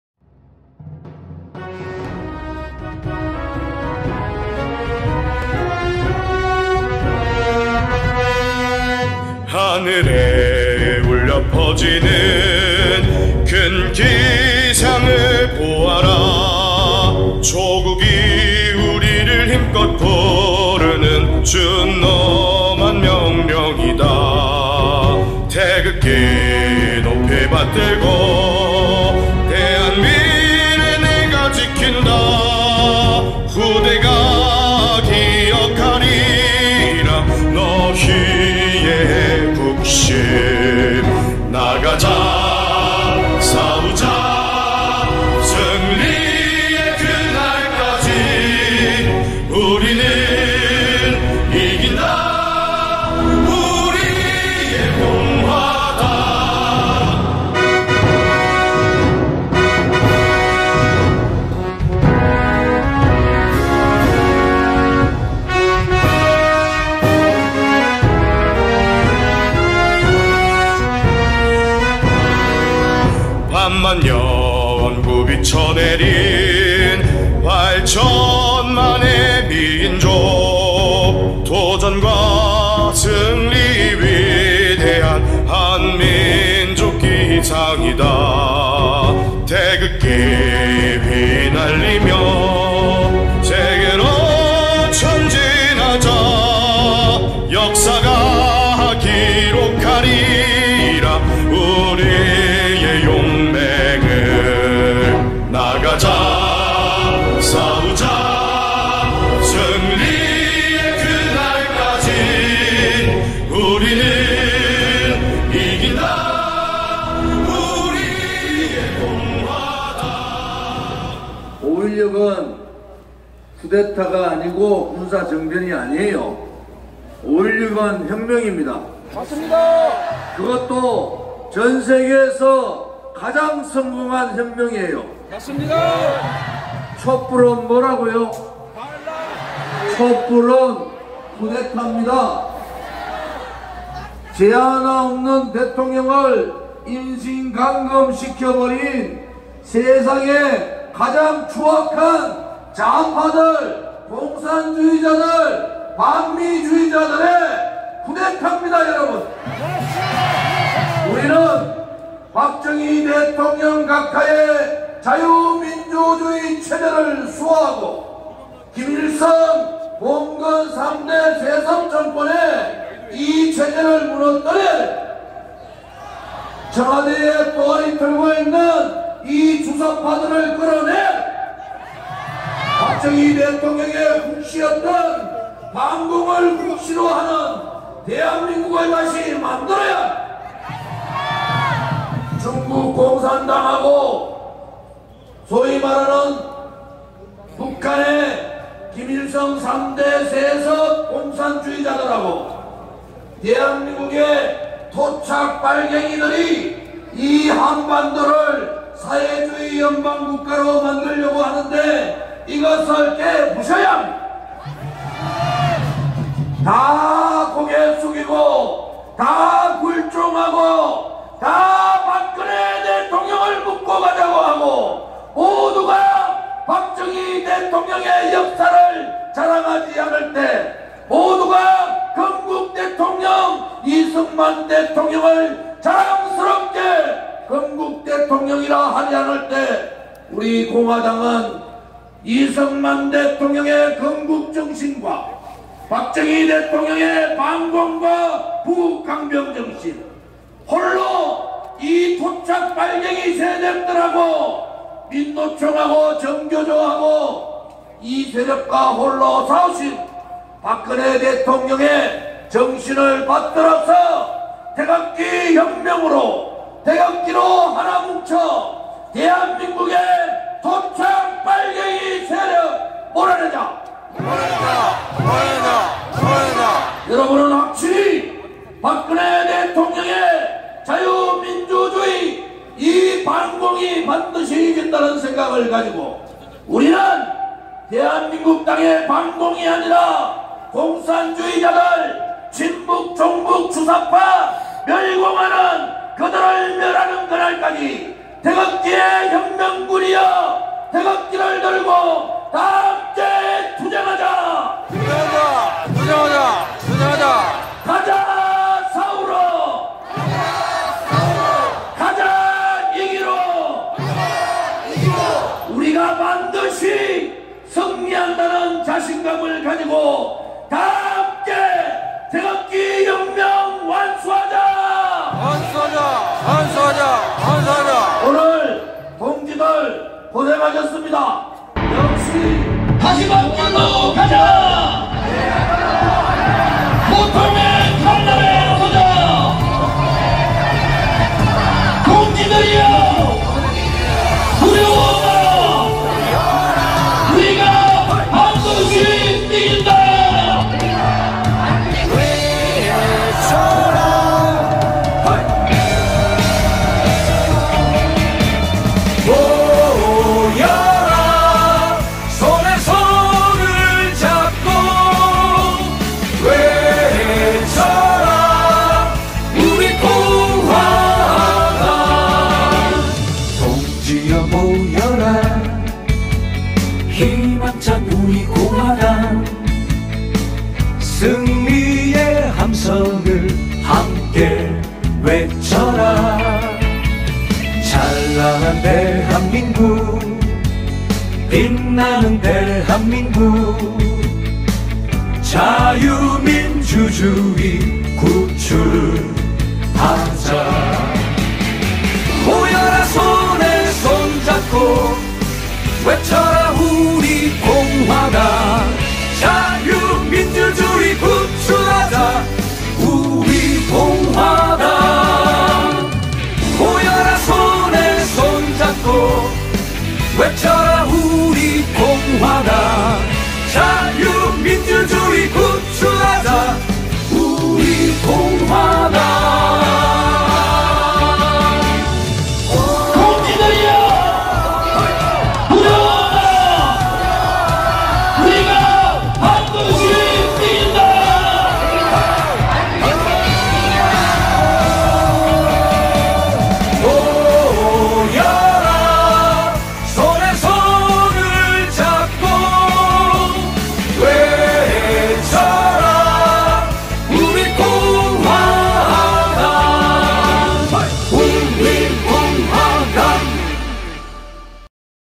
🌹2019. 10. 26 | 3부 집회 | 朴正熙 大統領 逝去 40 週忌 追慕 | 제151차 태극기집회🌹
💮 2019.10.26
💮3부 집회-오후 6시. 청운효자동 주민센터 앞(청와대 인근)